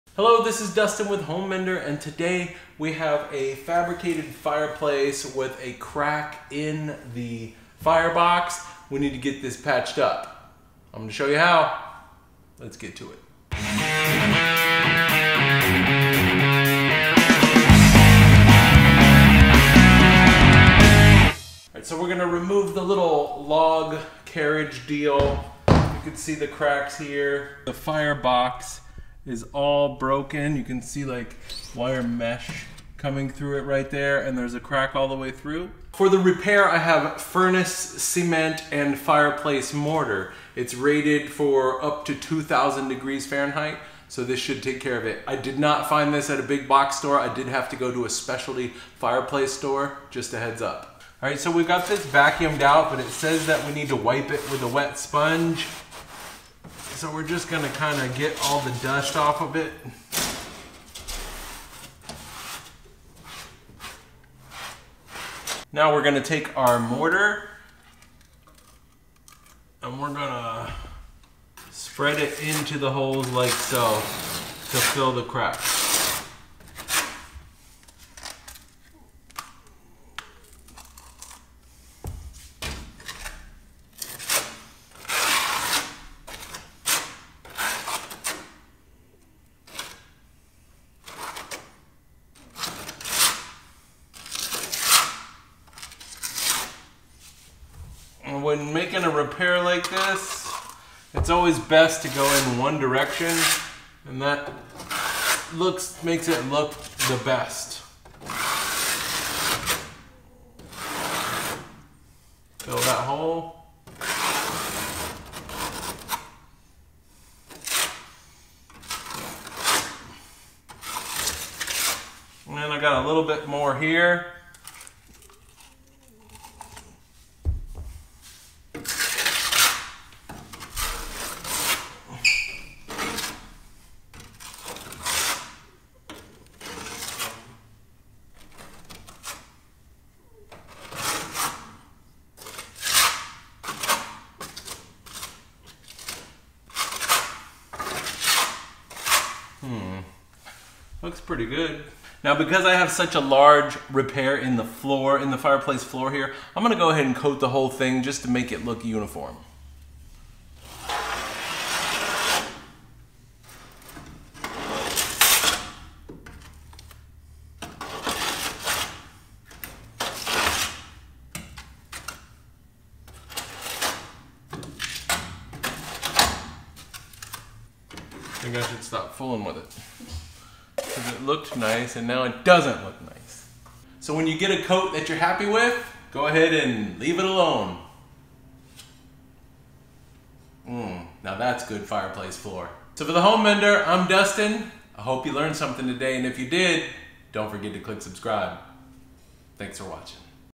how to fix firebox crack, cracked fireplace, fireplace repair
Hey Home Menders! A broken fireplace box can be an issue. especially if you are selling your home. Dustin shows us how to fix that firebox crack with ease. Clean and apply furnace cement and fireplace mortar , a compound that withstands heat ensuring a good fire place repair. You can do it! Easy!

🏠 HOME MENDERS E-COURSE
🛠 SAVE MONEY BY FIXING IT YOURSELF

1. Furnace cement and fireplace mortar
2. 6" mud knife
3. "home boy t shirt"

Do well but more importantly, do good  ❤️